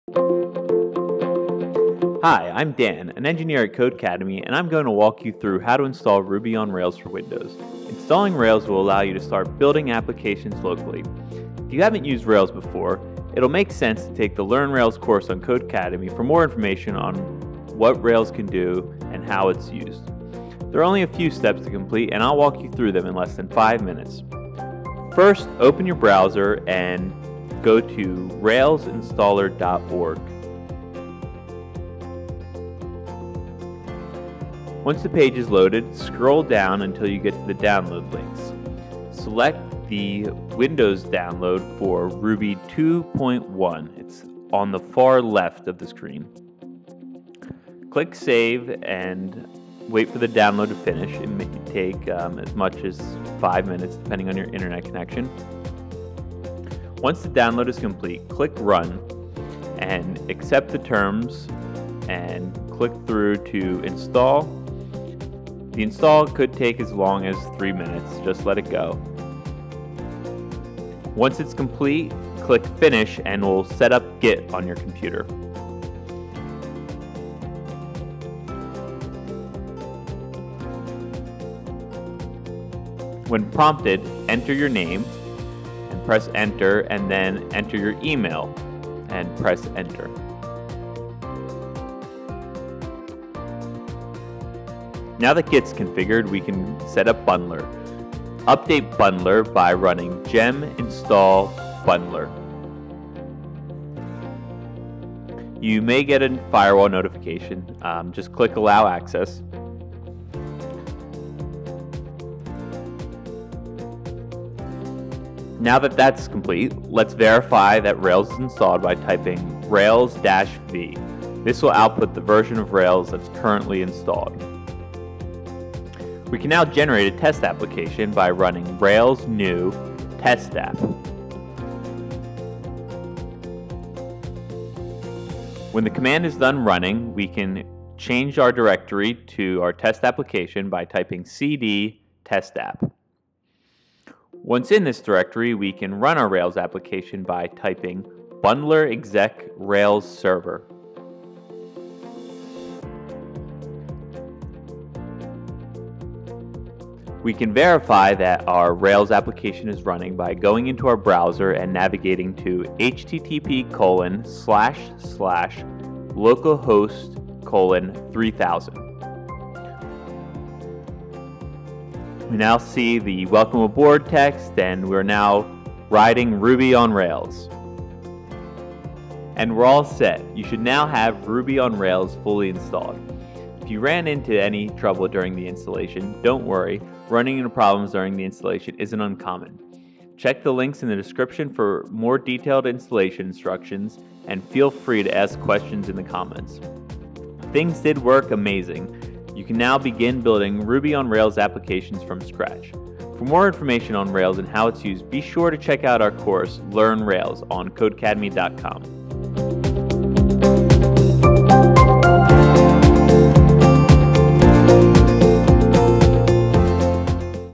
Install Ruby on Rails on Windows 7, 8 or 10 in 3 Minutes
As one of the most powerful ways to quickly develop web applications, Ruby on Rails has a high relevance and demand in today’s marketplace. We'll walk you through how to set up Ruby on Rails on your Windows machine in less than 5 minutes.

We'll be using to make the installation easier.